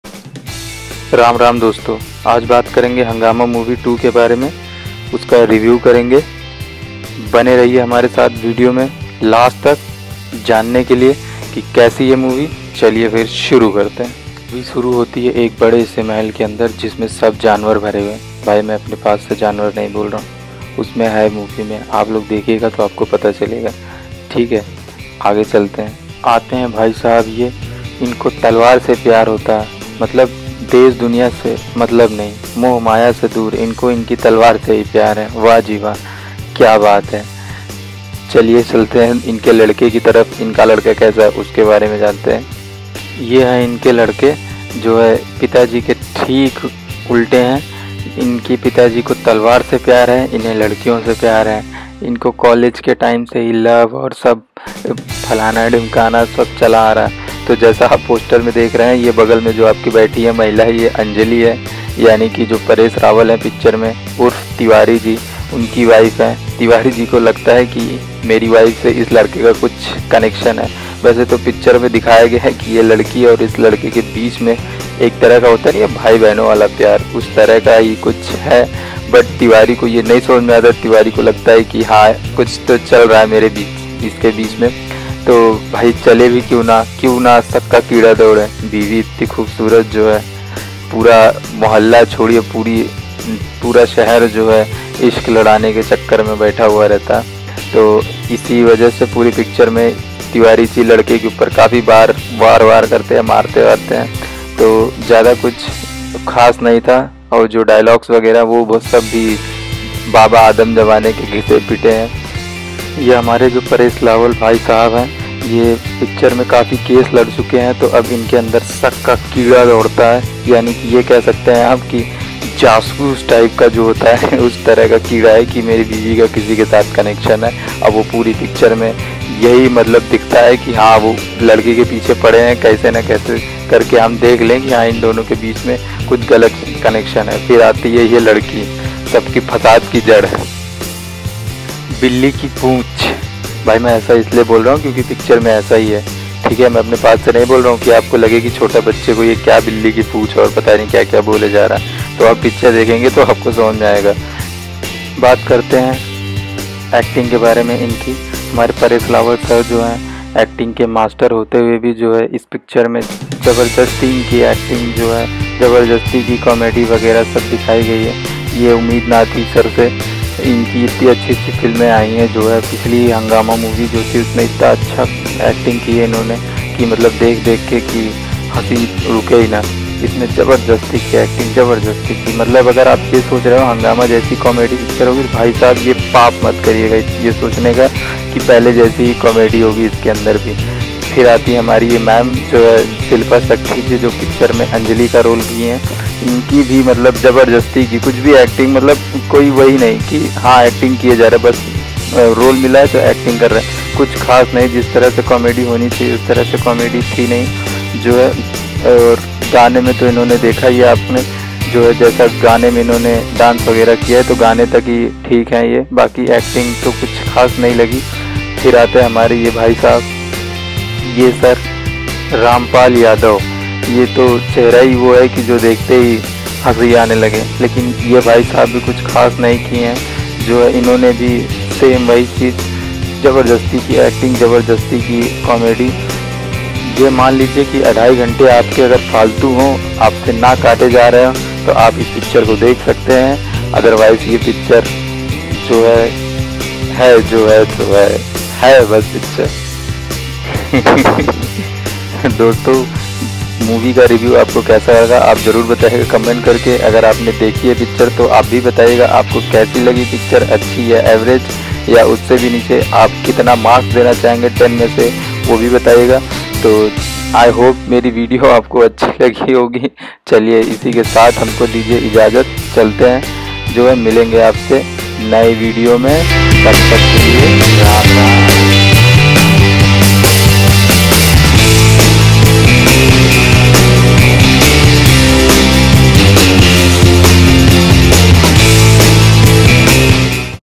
Review Hungama2 Movie
Movie khuch khas nhi h jitna umeed thi use jada nirasa mili h dekh ke movie ko phli hungama ne jitna hsaya ta utna hi is hugama 2 ne rulya h pta nhi kya soch ke bnai movie jabjsti ki comedy h jbajsti ki acting h ek bat jarur boluga ki khuch ho ya na ho pr movie dekhne ke bad dimak ka jhopda jarur  ban jaiga